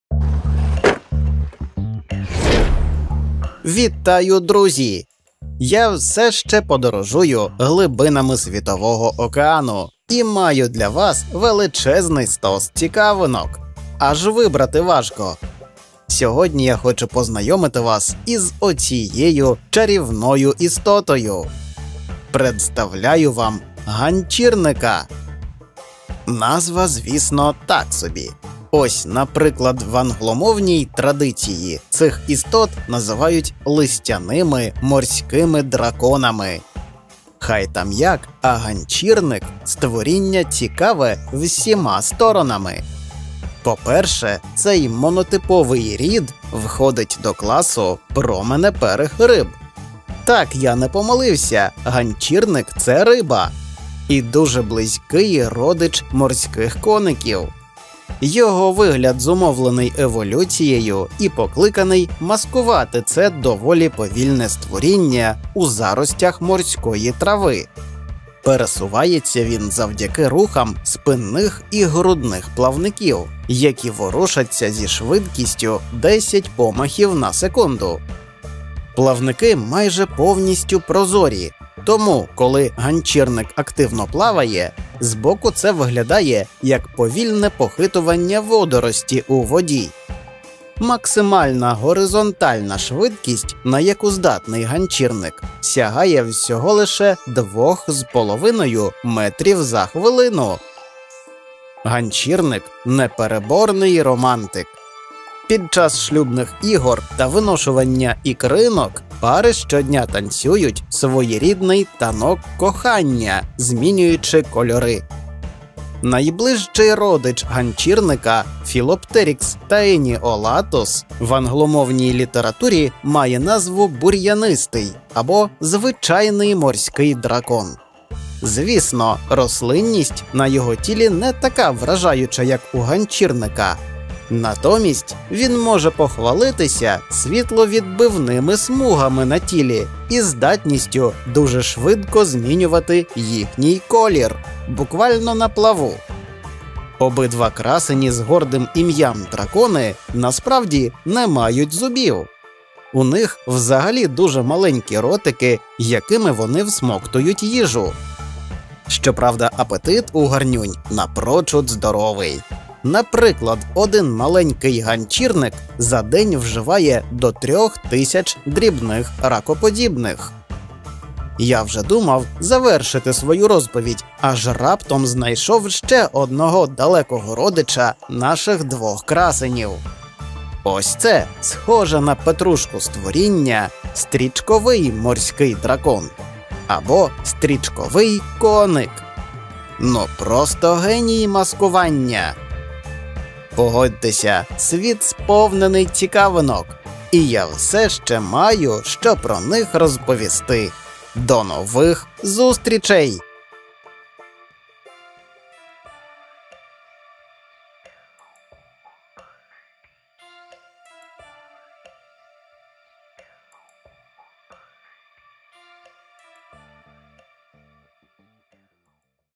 Дива природи. Явища навколо нас. Морські дракони: ганчірник та інші.
Природа — справжній художник із дійсно невичерпною фантазією. Уявіть, ось так, бува, бачиш, як під водою повільно пропливає гілочка морської рослини. Придивишся пильніше, а це, виявляється, і не рослина. А що ж? Подивіться нове відео! Там дійсно вражаючі подробиці!

«WilhelmagrüneSeepferdchen (1)» By Xocolatl (talk) 15:25, 28 April 2010 (UTC) - Self-photographed, Public Domain,